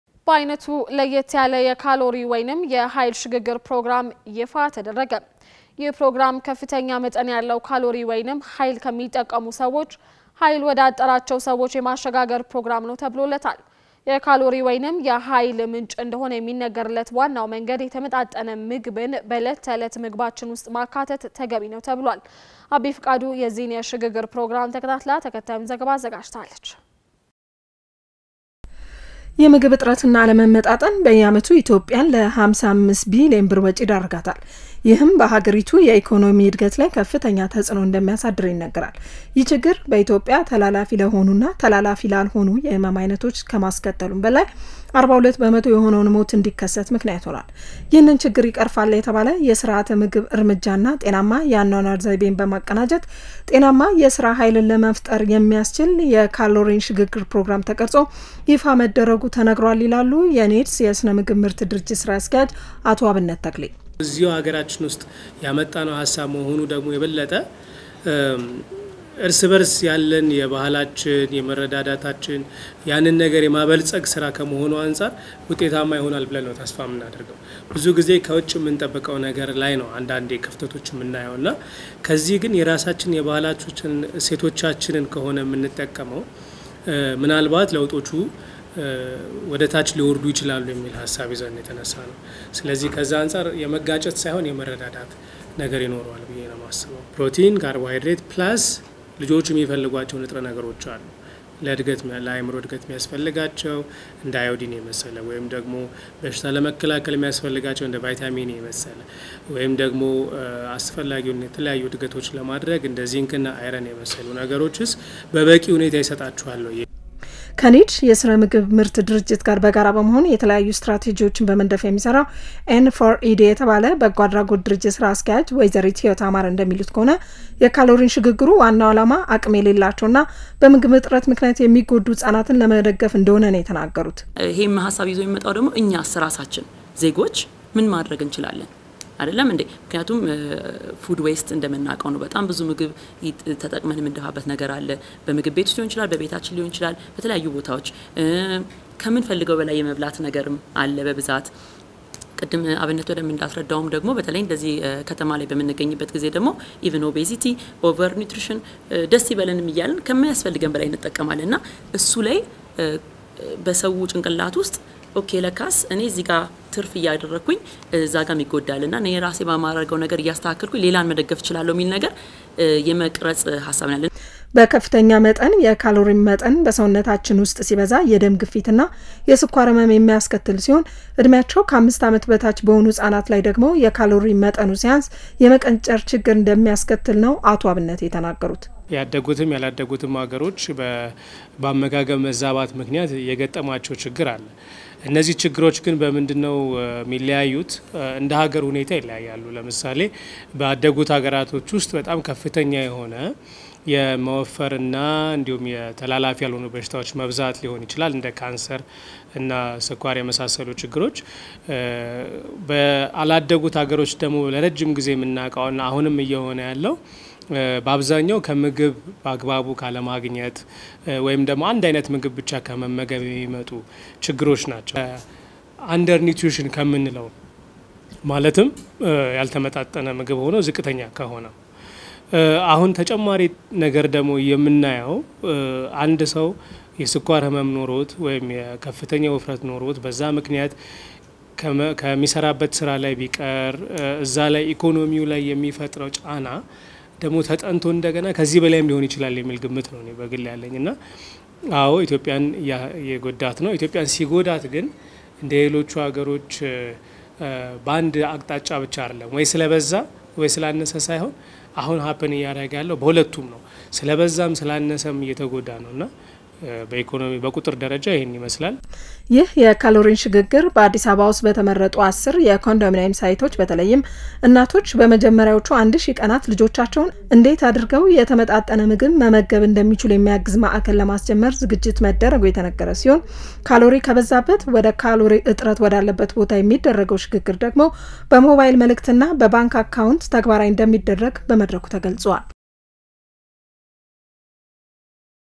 የኃይል ሽግግር ፕሮግራም መጀመር - News [Arts TV World]
በዓይነቱ ለየት ያለ የካሎሪ ወይም የኃይል ሽግግር ፕሮግራም ይፋ ተደረገ